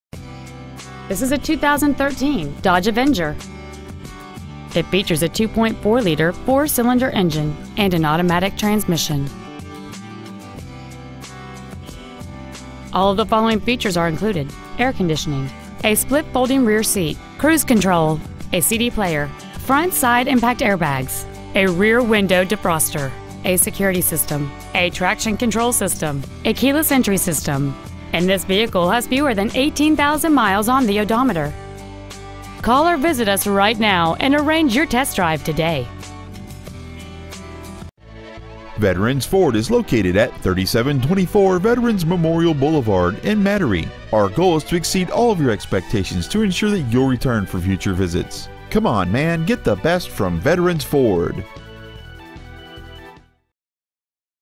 Used 2013 Dodge Avenger Baton Rouge LA
We have been honored to serve the Metairie LA area , we promise that your experience at our dealership will exceed your expectations ! Year : 2013 Make : Dodge Model : Avenger Engine : 2.4L I-4 cyl Trans . : Automatic Exterior : Black Miles : 17,100 Stock : P4214 Veterans Ford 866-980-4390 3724 Veterans Memorial Blvd Metairie , LA 70002